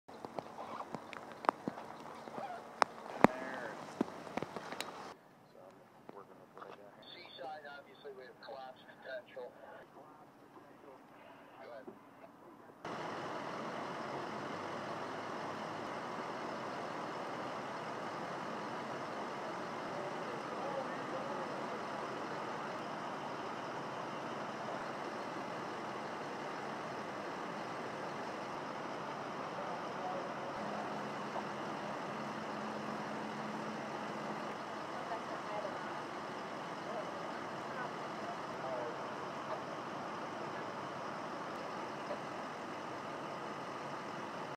Raw: Massillon fire
A large warehouse caught fire overnight in Massillon at National Feed Screw